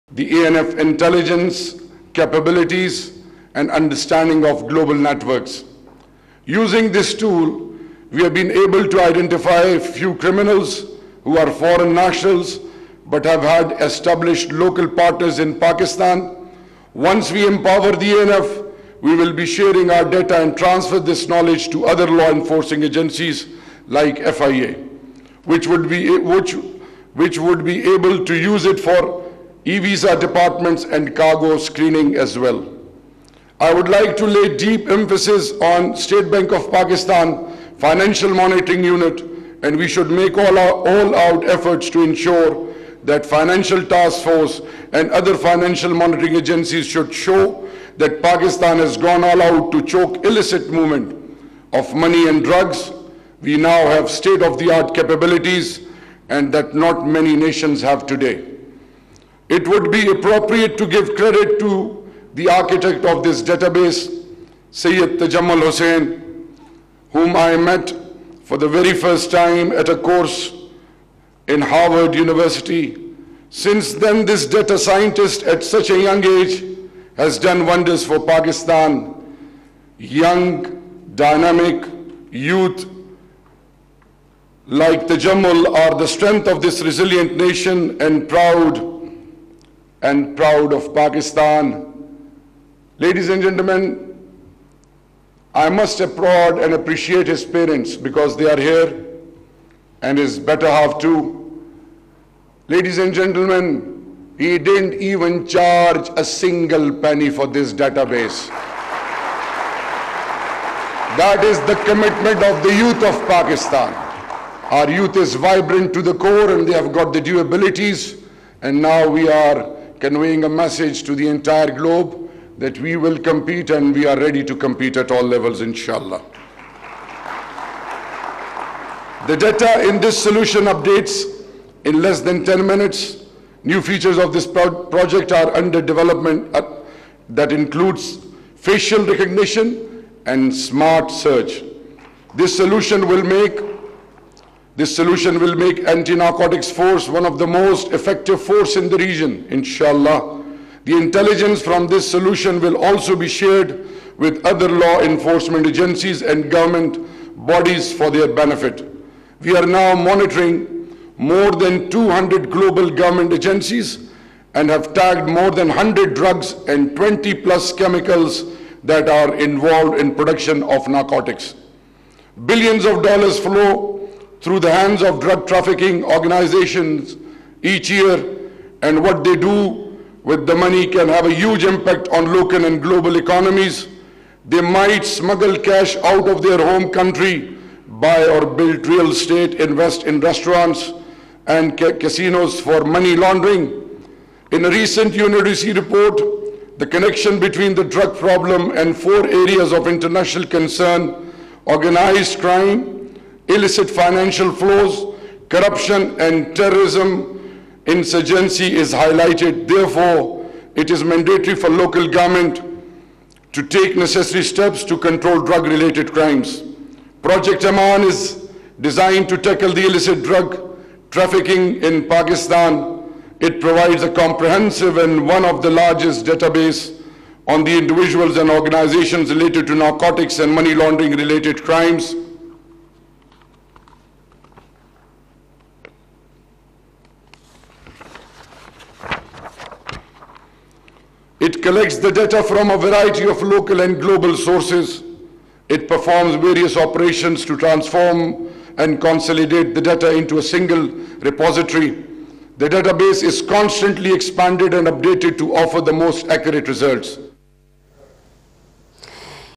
Minister of State for Interior Shehryar Afridi addresses ceremony in Islamabad | Samaa TV
Minister of State for Interior Shehryar Afridi addresses ceremony in Islamabad